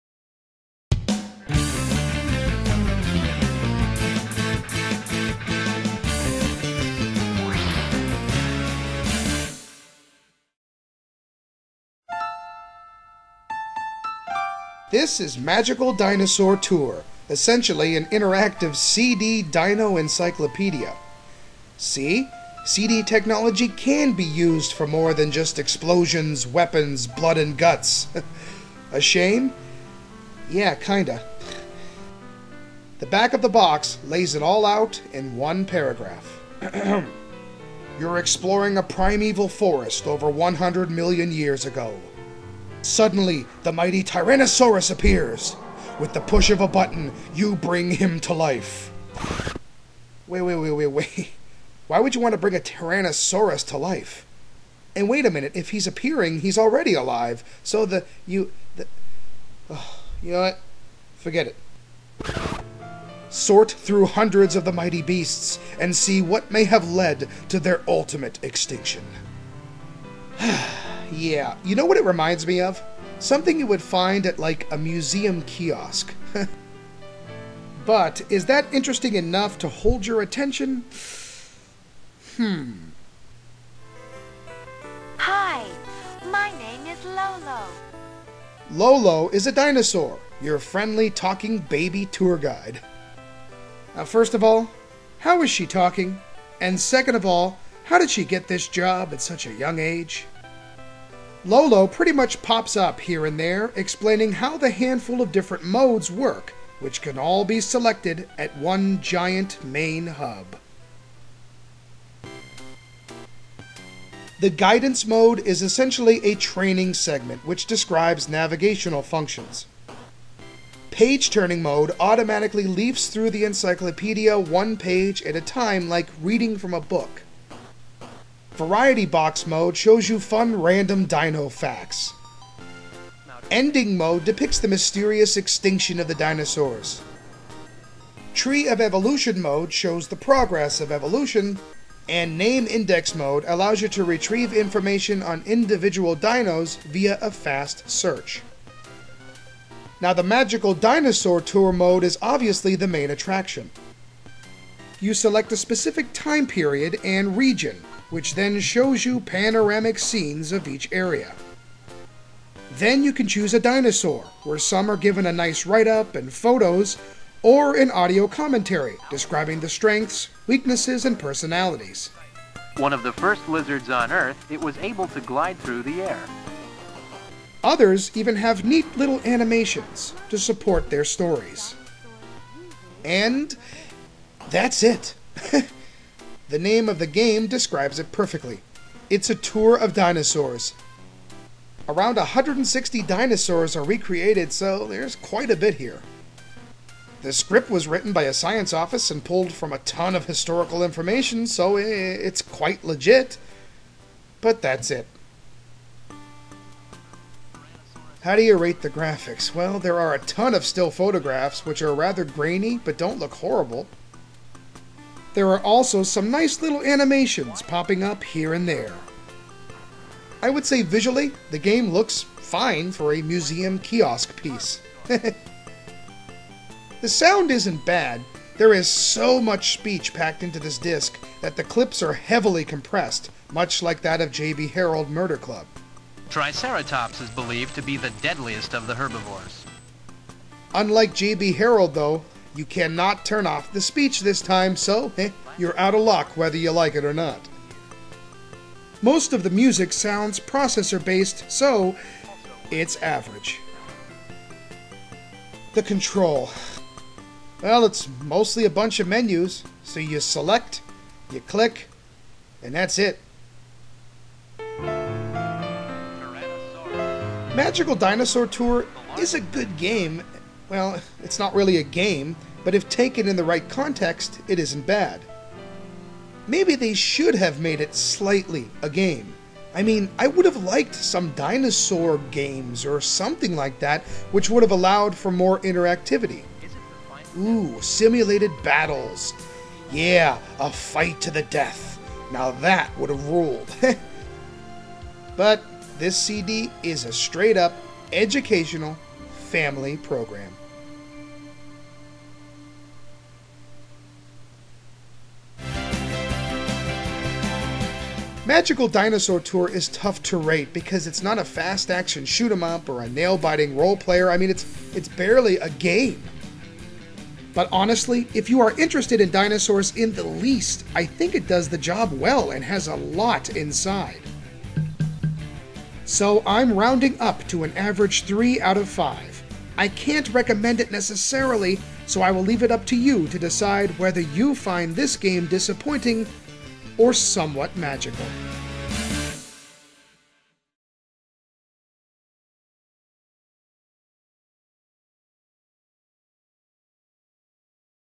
"Magical Dinosaur Tour" - Turbo Views 45 (TurboGrafx-16 / Duo game REVIEW!)
Turbo Views #45: video review of "Magical Dinosaur Tour" for the TurboGrafx 16 (CD-ROM) / Turbo Duo video-game system.